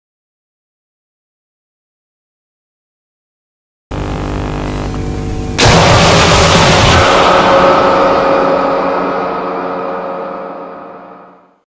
Spiney the Siamosaurus as Fa king sheet Jumpscare (Loud Jumpscare Warning)
Chicos hice un Muestro un nuevo Remake Llamado Spiney Pero Solo el Exe Jumpscare como Fa King Sheet Para episodios Perdidos en los Creditos

Note:I Got Error Cut scenes So We Fix this Jumpscare

Credits:@glitchyraptoranimationsmem3129 Or @glitchyraptoranimationsmem1298